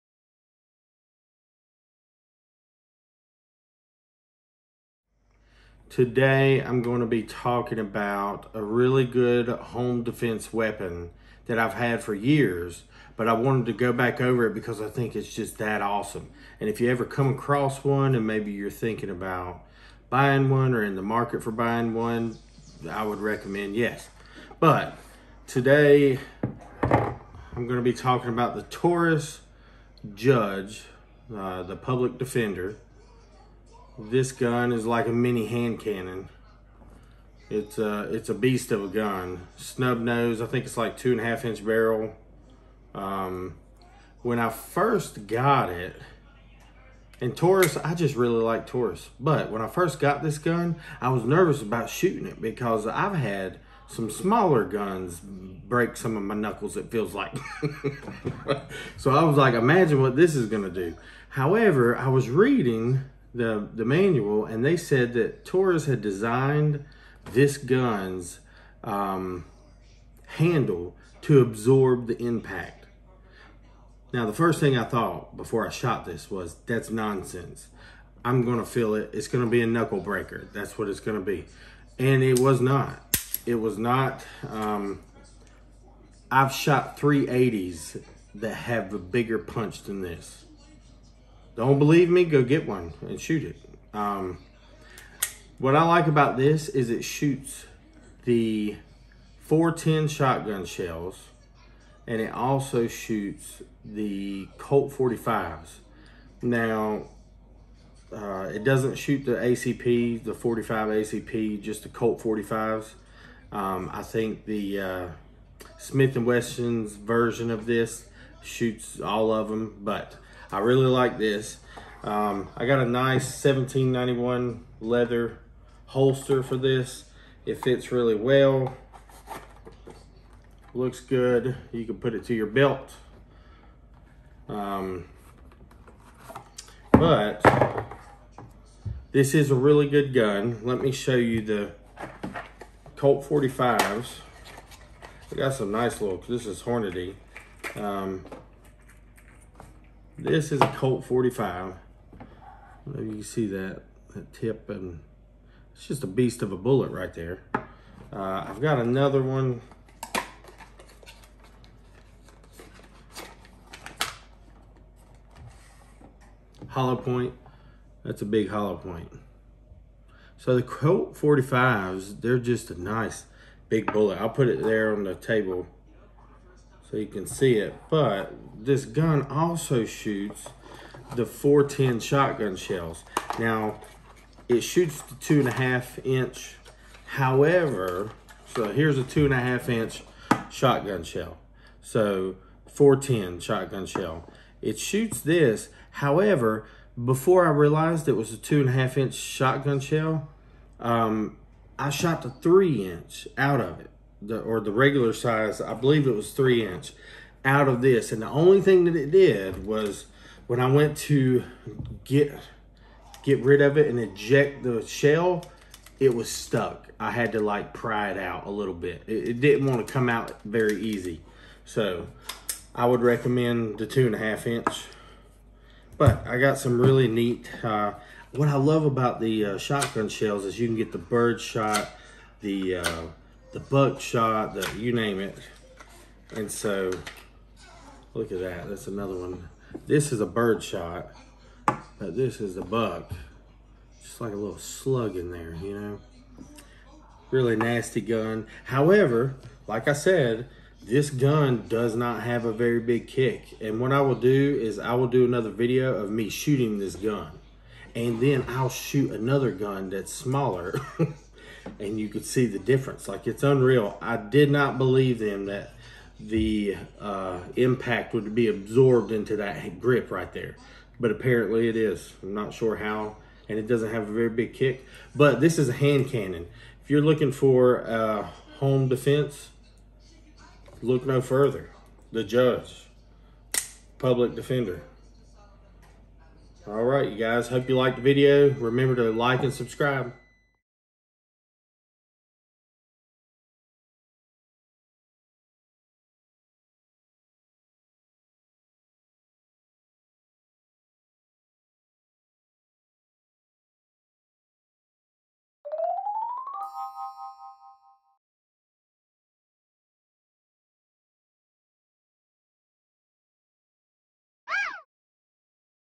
Taurus Public Defender | The Judge Review | CCFamily
Dad w/CCFamily gives a review of The Judge by Taurus. The Public Defender. Great home defense weapon. Check this hand cannon out. CCFamily…